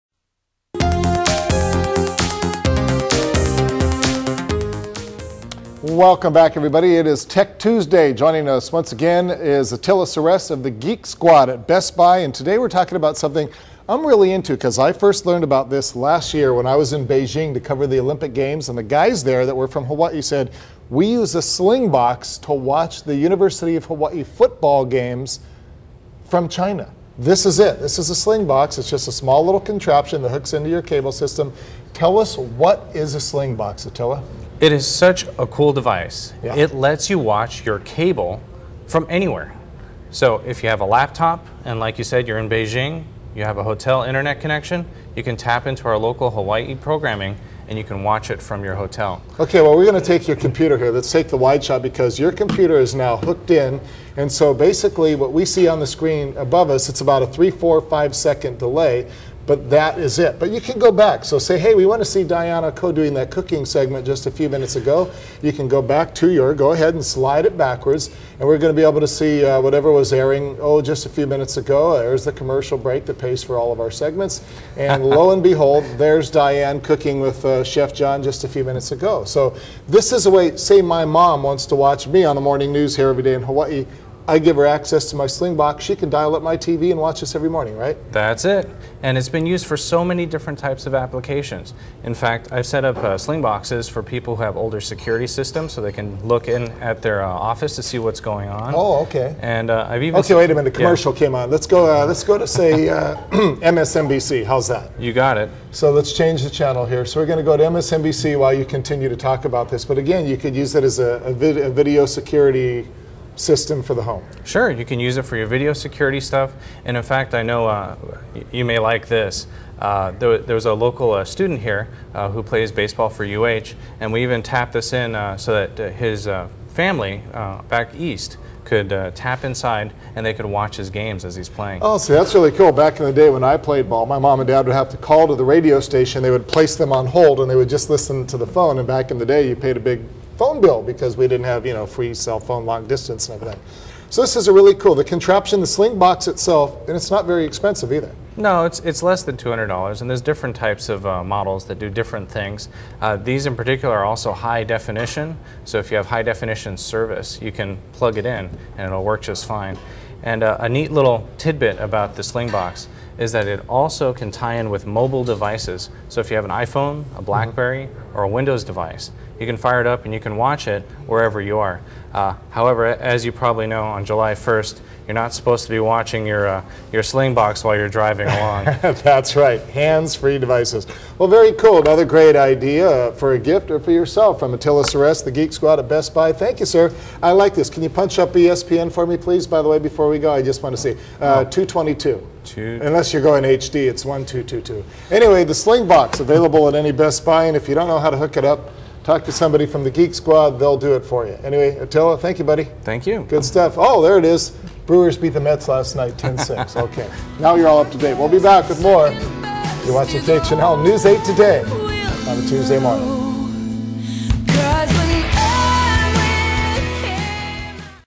6/30/09 Tech Tuesday on Slingbox
Slingbox Made Simple -

There's a good chance you've heard the word Slingbox, maybe in passing or from another co-worker. Perhaps when someone tried to tell you how it works, you got confused and never thought about it again. However, the Slingbox and its related products have been around for some years now and they compose a body of legitimately useful products.

The Slingbox is a media extender, kind of like a long distance remote control that lets you use your laptop to watch your home theater equipment. It connects to your cable box, satellite dish or even your DVR and then lets you connect and control it over the Internet. Simply put, it lets you watch your cable TV from wherever you are.

How can I use it?

Although it's a little difficult to understand, it has many uses. For example, if you're on vacation and want to watch some of our local Hawaii programming, you can use your laptop to go online and watch your Slingbox. I've set up a few Slingboxes for people and seen their uses first hand. For example, I had one customer who liked to visit Japan often and wanted to be able to watch the local Hawaii news from their hotel. I set up another for a business with an older surveillance system so that they could monitor their office from home. I even set one up for a UH student who plays baseball so that their family on the mainland could watch him play games on our local television.

One thing that's great about Sling Media, the company that makes the Slingbox, is how they really try to be practical about their products. For example, there's Slingplayer software available for Blackberry, Windows Mobile devices and now even the iPhone so that you can watch programming on the go. They even have a product called the Slingcatcher that connects to a TV, so you can watch your Slingbox on another TV in your house or say at your vacation home on the Big Island.

Final Note

In short, the Slingbox line of products are solid and competitors haven't been able to take away Slingbox's top spot. It's a worthwhile addition to any home theater system and useful for watching your TV or recorded content when you're not home. Just don't watch it while driving or get caught watching it at work.

-Attila Seress